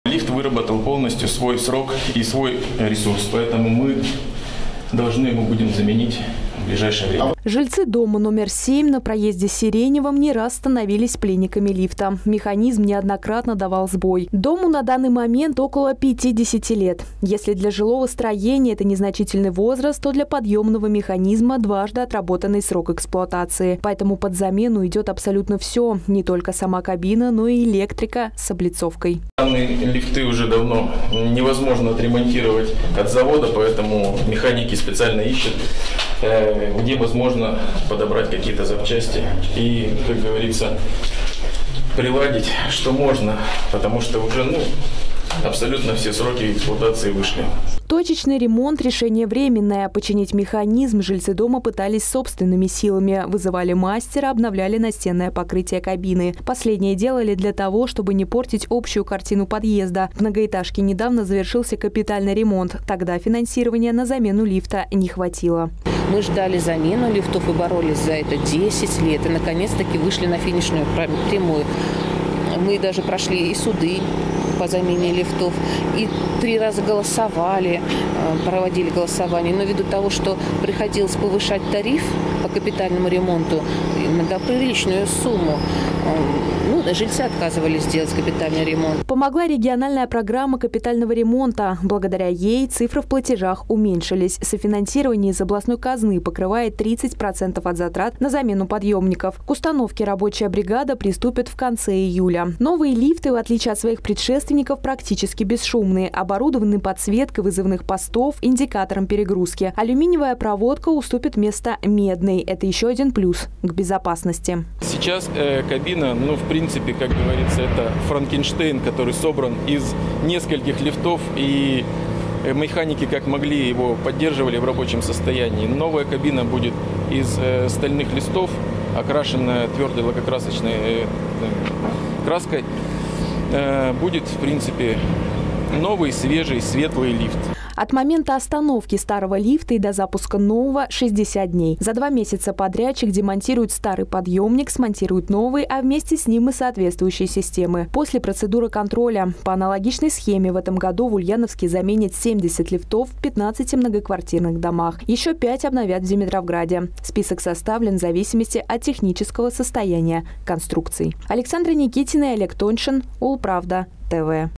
Долой Франкенштейна. В Ульяновске и в Димитровграде начали менять лифты
В этом году в Ульяновске и Димитровграде обновят 75 лифтов – пять подъемных механизмов в городе атомщиков и 70 в областной столице. Список составлен на основе заявок от жильцов. Одна из них поступила от ульяновских активистов дома №7  на проезде Сиреневом. Здесь лифт отслужил два срока эксплуатации – 50 лет, взамен положенных 25.